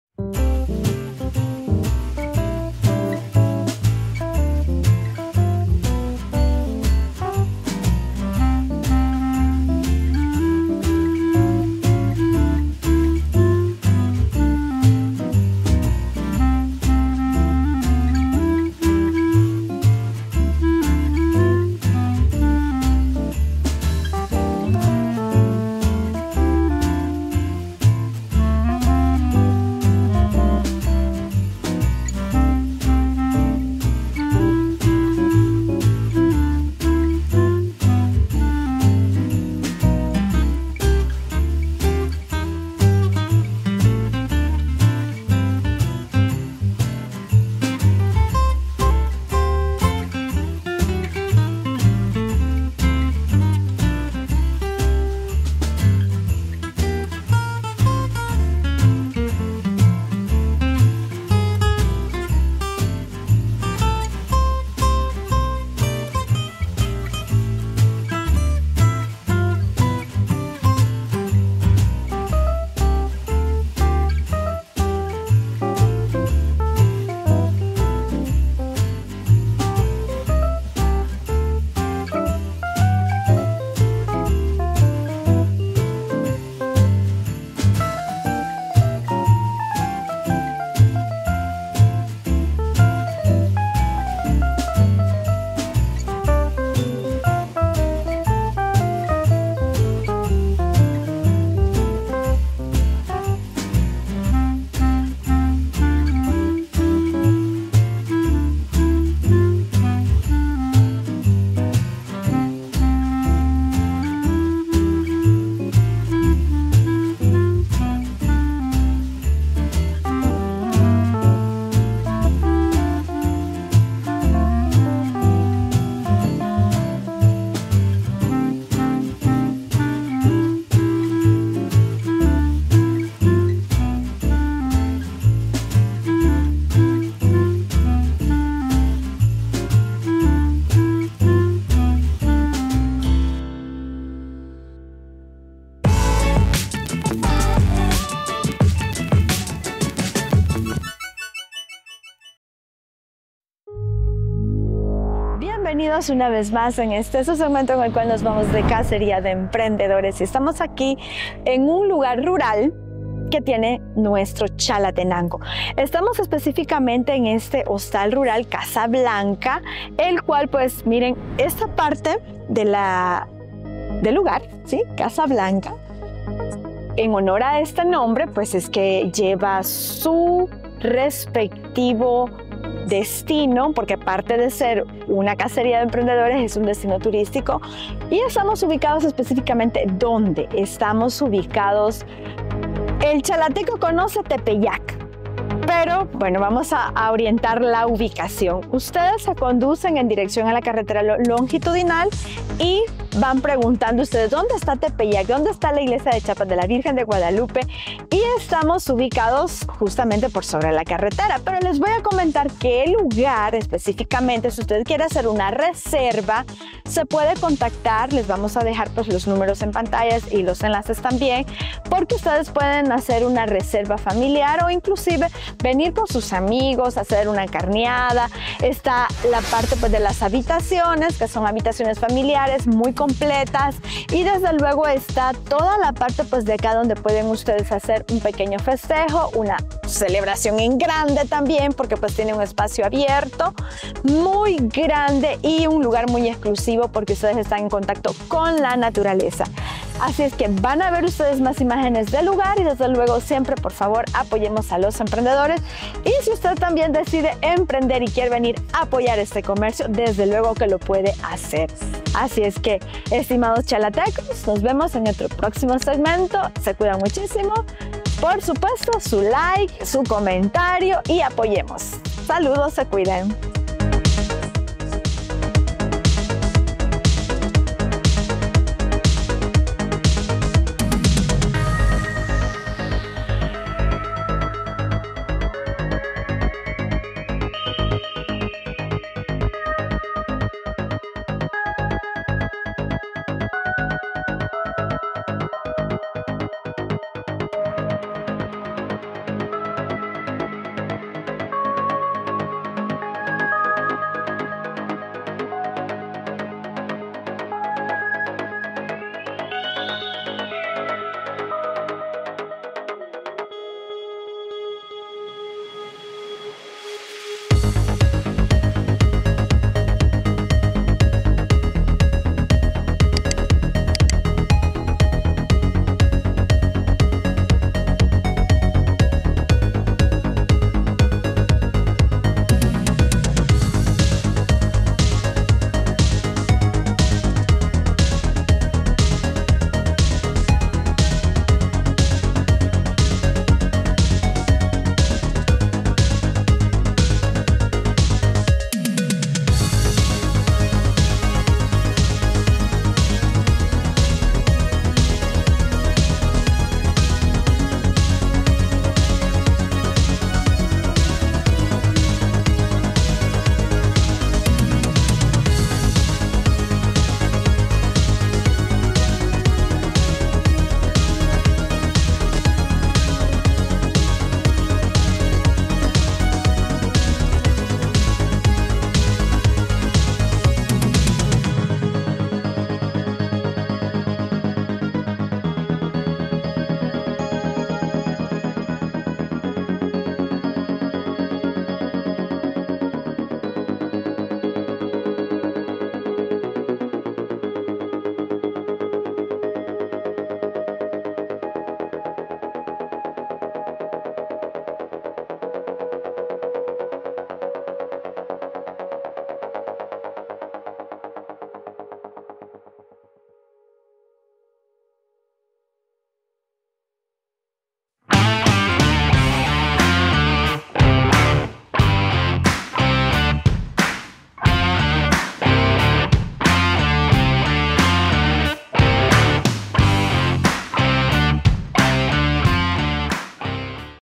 Hostal Rural Casa Blanca: un oasis de tranquilidad y naturaleza en Chalatenango
¿Te gustaría escapar del estrés de la ciudad y disfrutar de un lugar rodeado de naturaleza y tranquilidad? Entonces, no te pierdas este vídeo sobre el Hostal Casa Blanca, un alojamiento ecológico y acogedor ubicado en Chalatenango, El Salvador. En este vídeo, te mostraremos las instalaciones del hostal y las actividades que puedes realizar en sus alrededores. Descubre por qué el Hostal Casa Blanca es un oasis de paz y armonía en el corazón de Chalatenango.